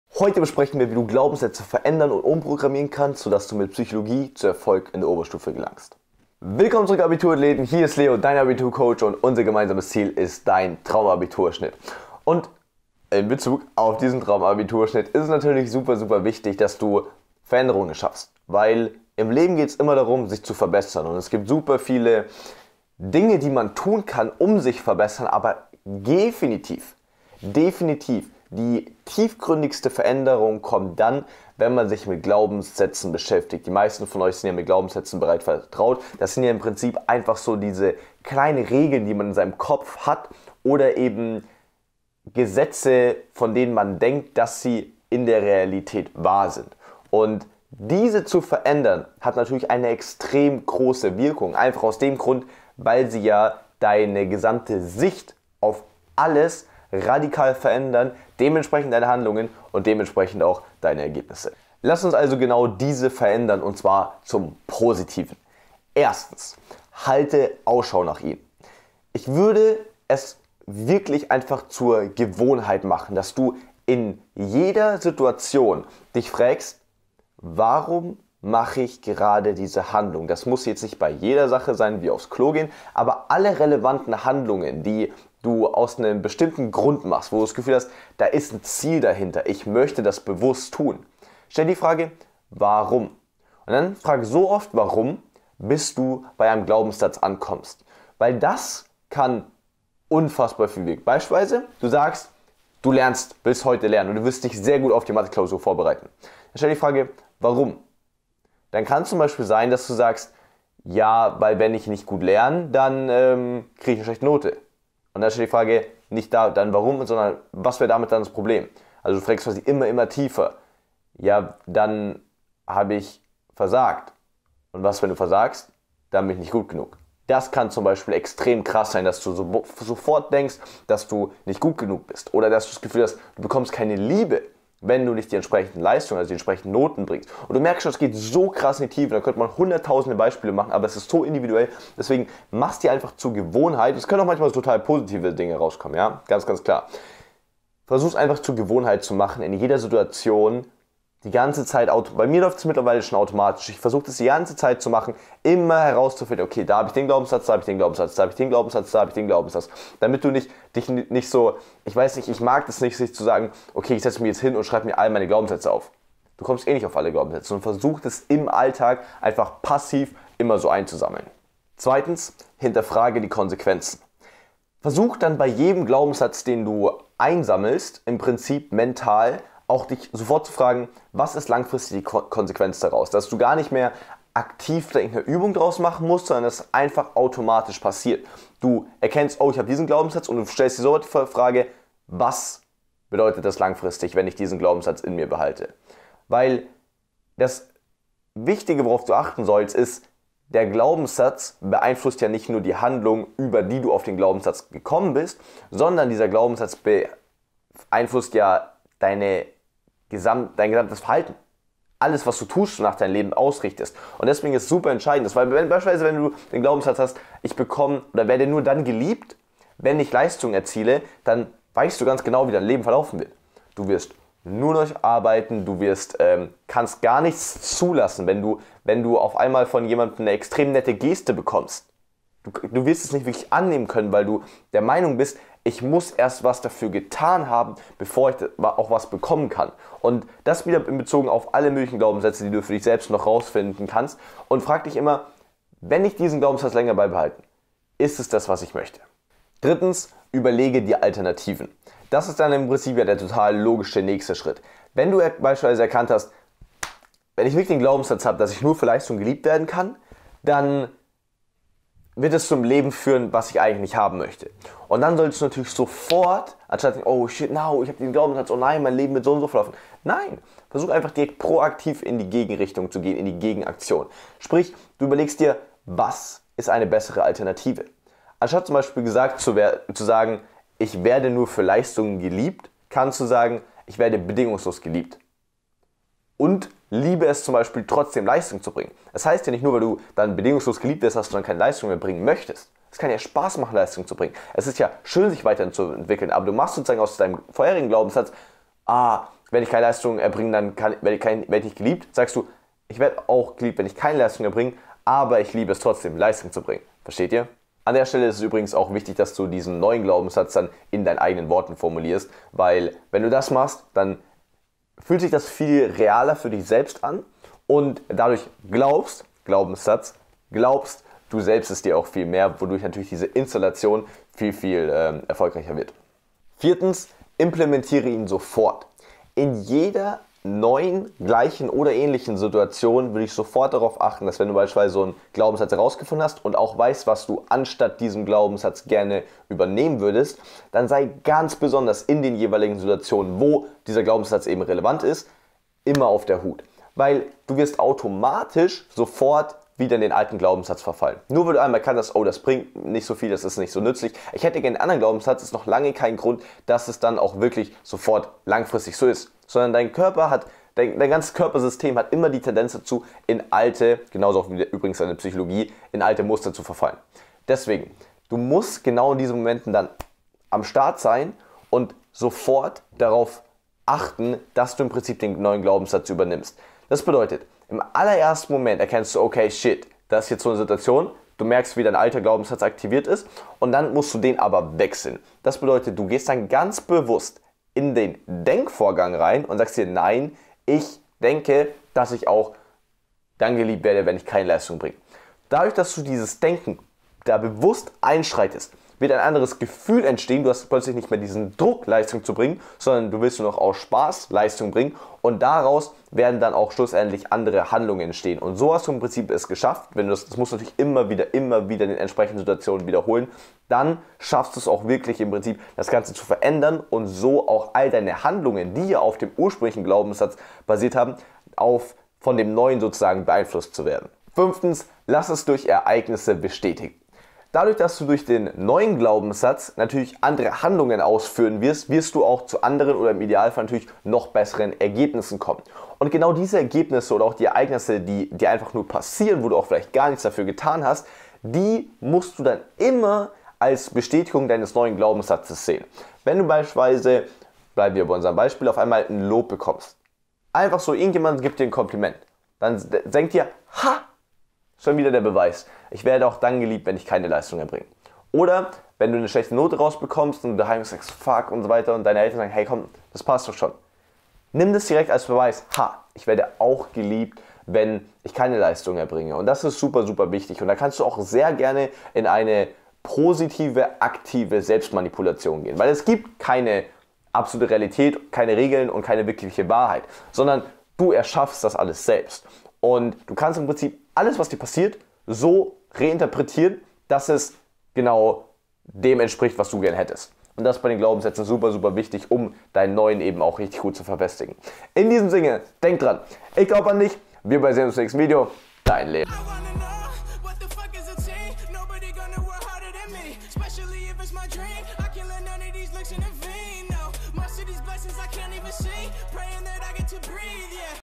Glaubenssätze verändern & dauerhaft umprogrammieren! (Erfolg-Psychologie in der Oberstufe)
Glaubenssätze verändern, erkennen, auflösen und dauerhaft umprogrammieren. Heute sprechen wir über negative Glaubenssätze und wie du sie mit Erfolg-Psychologie in der Oberstufe finden und ändern kannst. ENJOY :)

- Euer Leo